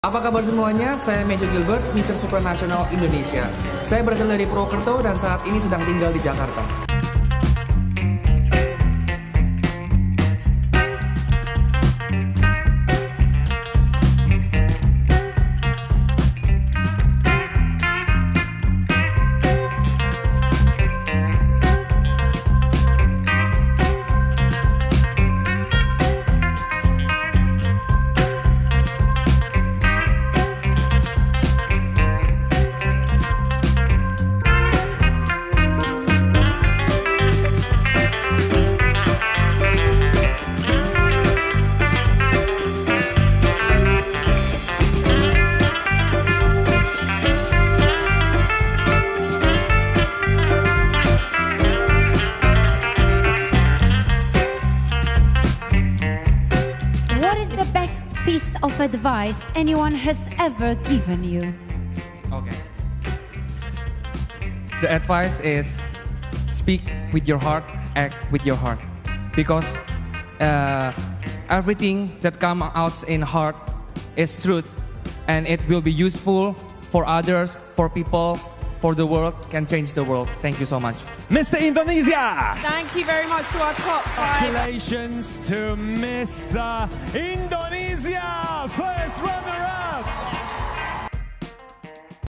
Performance of Matthew Gilbert at Mister Supranational Final Show || Mister Supranational 2022
Matthew Gilbertw is Mister Supranational Indonesia 2022 and The 1st Runner Up Mister Supranational 2022. Congratulations For Him. Indonesian So Pround of You.